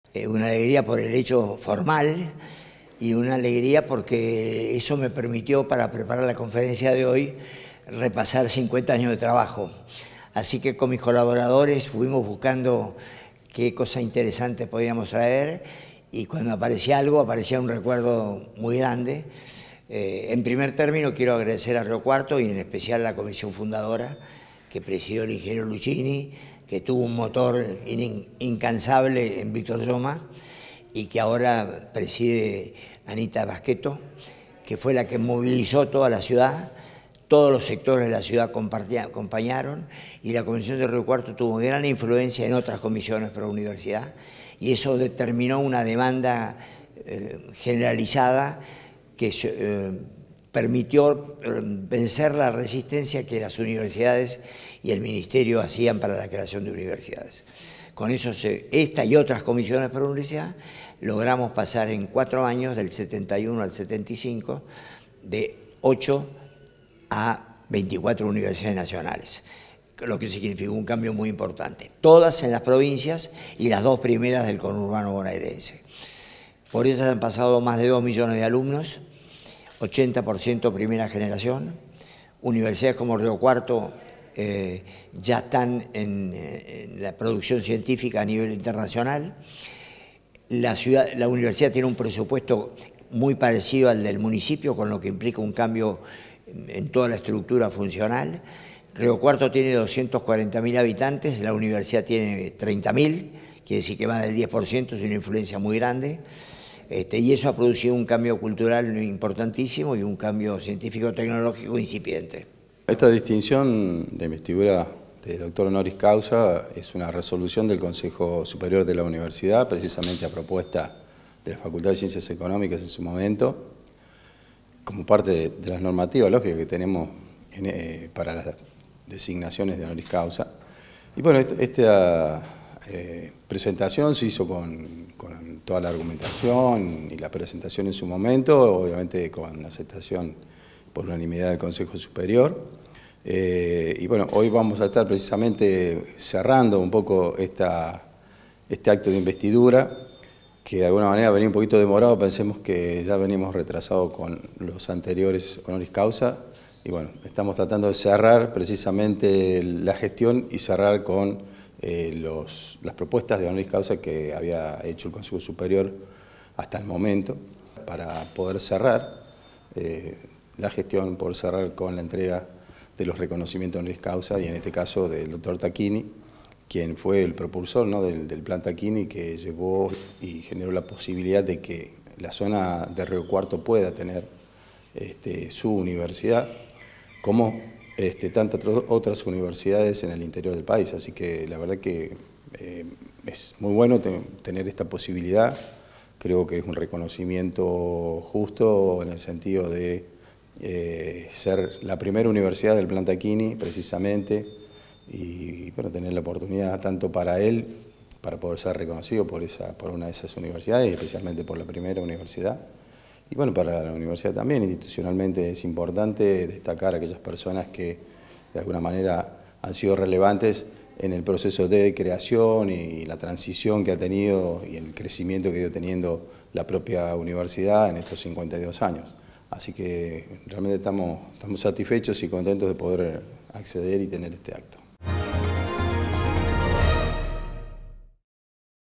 MÁXIMA DISTINCIÓN: LA UNRC ENTREGÓ EL TÍTULO DR. HONORIS CAUSA A ALBERTO TAQUINI (h)
ALBERTO TAQUINI (H) – IMPULSOR PLAN NUEVAS UNIVERSIDADES
ROBERTO ROVERE – RECTOR